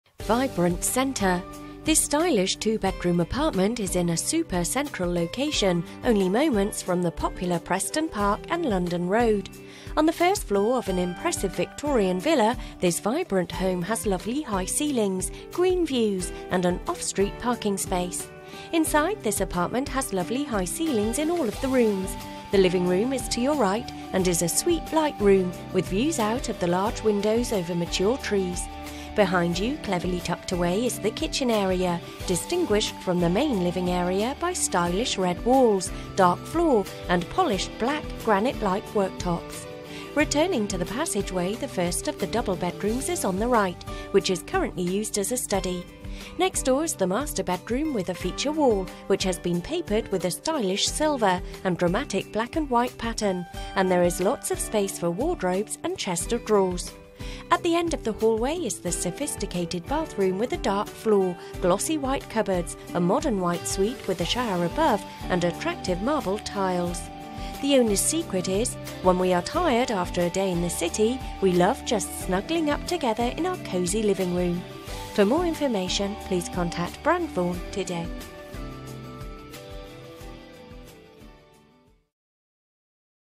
Flat on Stanford Avenue, Preston Park, Brighton, East Sussex
VIBRANT • CENTRE

This stylish two bedroom apartment is in a super, central location only moments from the popular Preston Park and London Road. On the first floor of an impressive Victorian Villa, this vibrant home has lovely high ceilings, green views and an off street parking space.

In brief:
Style: First floor apartment
Bedrooms: 2 double
Living rooms: 1 open plan
Area: 482 SQ. FT. 44.8 SQ. M
Location: Preston Park
Parking: Off street parking